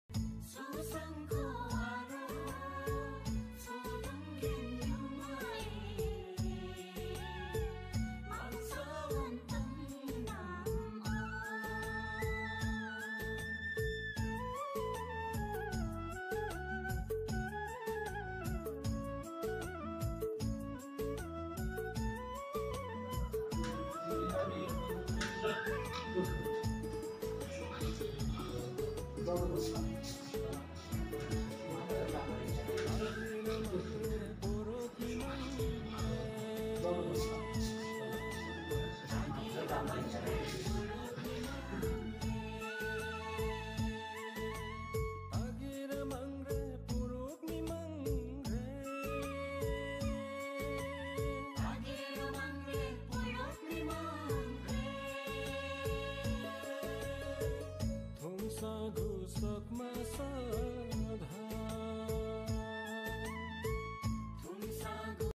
Voluntarily Blood Donation 😮
On the auspicious occasion of 18th Spiritual Birth Anniversary of Her Holiness The Sikkum Siyong Sepsing Yumamang at the district hospital Gyalshing west Sikkim.